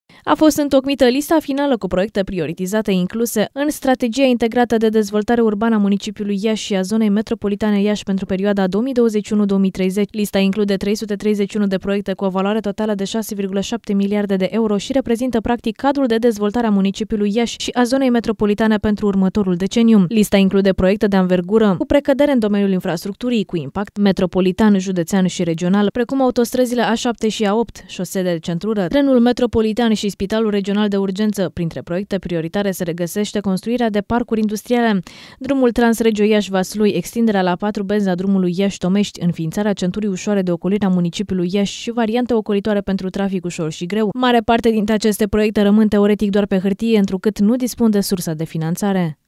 Proiecte în valoare de 6,7 miliarde de euro pentru Zona Metropolitana Iasi
A fost întocmita lista finală cu proiecte prioritizate incluse în Strategia Integrată de Dezvoltare Urbană a Municipiului Iași și a Zonei Metropolitane Iași pentru perioada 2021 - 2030. Lista include 331 de proiecte cu o valoare totală de 6,7 miliarde de euro și reprezintă, practic, cadrul de dezvoltare a Municipiului Iași și a Zonei Metropolitane pentru următorul deceniu.